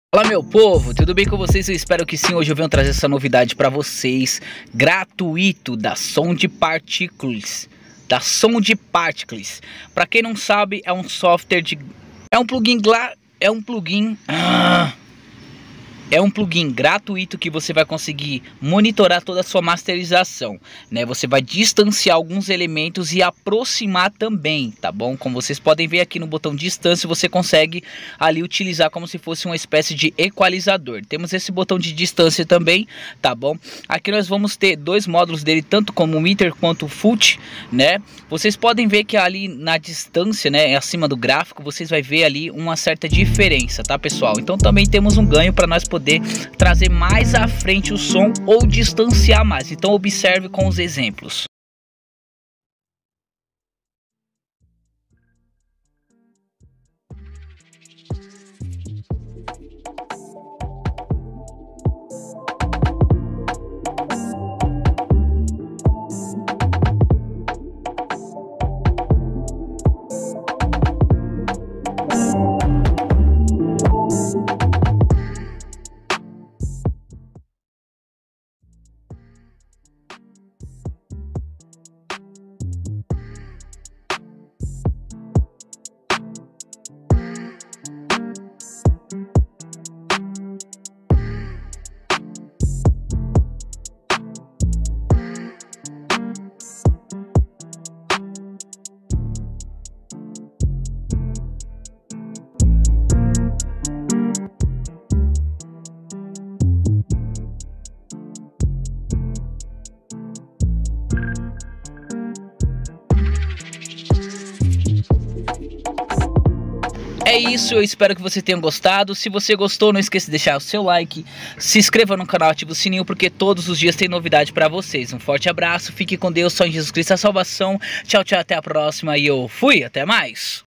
SOUND PARTICULES AIR | APROXIME SUA MIXAGEM | GRÁTIS POR TEMPO LIMITADO
OLÁ SEJA BEM VINDOS(AS) AO MEU CANAL NO VIDEO DE HOJE VENHO LHE MOSTRAR UM PLUGIN INCRÍVEL , PARA FAZER SUAS MELODIAS .

Use o CUPOM: DJEMENOBEAT

📧 Contato para Mixagem, Workshops ou Consultoria 📧